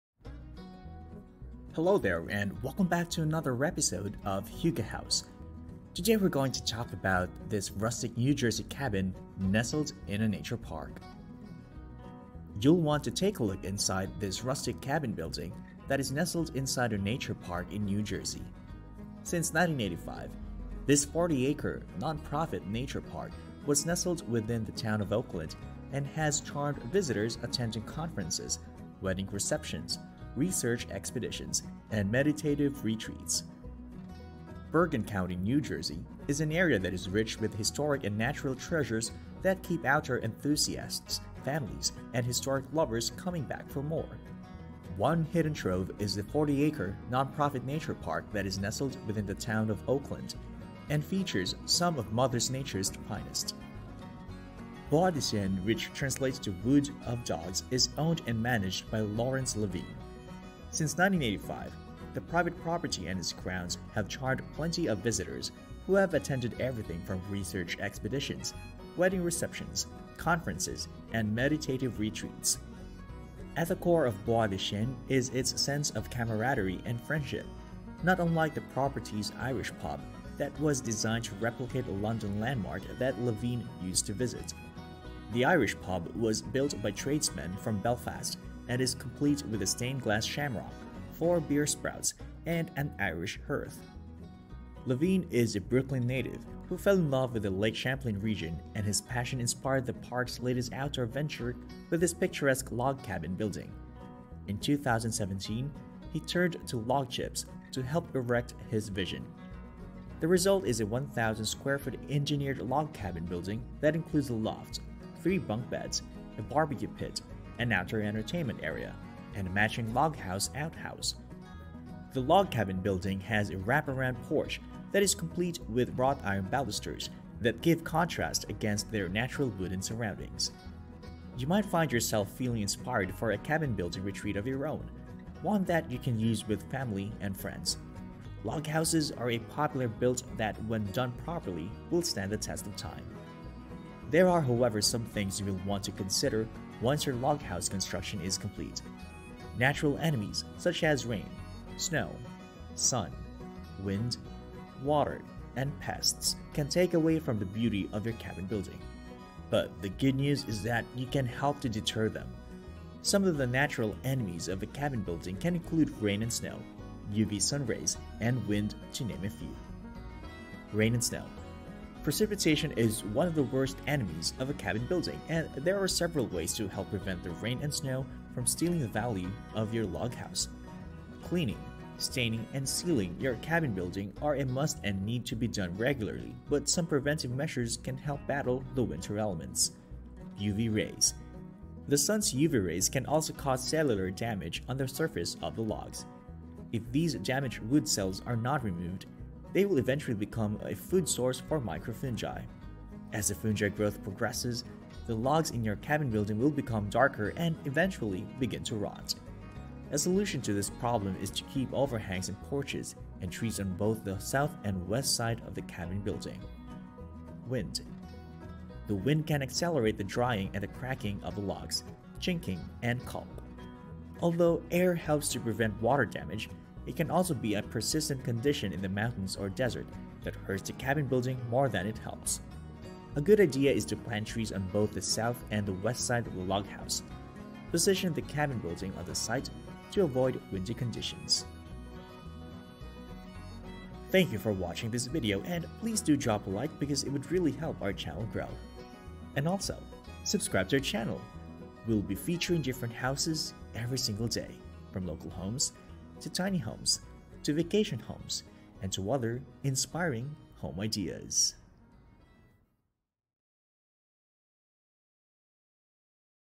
A Rustic New Jersey Cabin Nestled In A Nature Park
You'll want to take a look inside this rustic cabin building that is nestled inside a nature park in New Jersey. Since 1985, this 40-acre, non-profit nature park was nestled within the town of Oakland and has charmed visitors attending conferences, wedding receptions, research expeditions, and meditative retreats. Bergen County, New Jersey, is an area that is rich with historic and natural treasures that keep outdoor enthusiasts, families, and history lovers coming back for more. One hidden trove is the 40-acre, non-profit nature park that is nestled within the town of Oakland, fand features some of Mother Nature’s finest.

Bois de Chiens which translates to Woods of Dogs is owned and managed by Laurence Levine. Since 1985, the private property and its grounds have charmed plenty of visitors who have attended everything from research expeditions, wedding receptions, conferences, and meditative retreats. At the core of Bois, de Chiens is its sense of camaraderie and friendship, not unlike the property’s Irish pub, that was designed to replicate a London landmark that Levine used to visit. The Irish pub was built by tradesmen from Belfast and is complete with a stained glass shamrock, four beer spouts, and an Irish hearth.

Levine is a Brooklyn native who fell in love with the Lake Champlain Region, and his passion inspired the park’s latest outdoor venture with this picturesque log cabin building. In 2017, he turned to Log Chips, to help erect his vision. The result is a 1,000-square-foot, engineered log cabin building that includes a loft, three bunk beds, a barbecue pit, an outdoor entertainment area, and a matching log house outhouse. The log cabin building has a wrap-around porch that is complete with wrought-iron balusters that give contrast against their natural wooden surroundings.

You might find yourself feeling inspired for a cabin building retreat of your own, one that you can use with family and friends. Log houses are a popular build that when done properly will stand the test of time. There are, however, some things you will want to consider once your log house construction is complete. Natural enemies such as rain, snow, sun, wind, water, and pests can take away from the beauty of your cabin building, but the good news is that you can help to deter them. Some of the natural enemies of a cabin building can include rain and snow, UV sun rays, and wind to name a few.

Rain and snow. Precipitation is one of the worst enemies of a cabin building, and there are several ways to help prevent the rain and snow from stealing the value of your log house. Cleaning, staining and sealing your cabin building are a must and need to be done regularly but some preventative measures can help battle the winter elements.

UV Rays. The sun’s UV rays can also cause cellular damage on the surface of the logs. If these damaged wood cells are not removed, they will eventually become a food source for microfungi. As the fungi growth progresses, the logs in your cabin building will become darker and eventually begin to rot. A solution to this problem is to keep overhangs and porches, and trees on both the south and west side of the cabin building.

Wind. The wind can accelerate the drying and the cracking of the logs, chinking, and caulk. Although air helps to prevent water damage, it can also be a persistent condition in the mountains or desert that hurts the cabin building more than it helps. A good idea is to plant trees on both the south and the west side of the log house. Position the cabin building on the site to avoid windy conditions.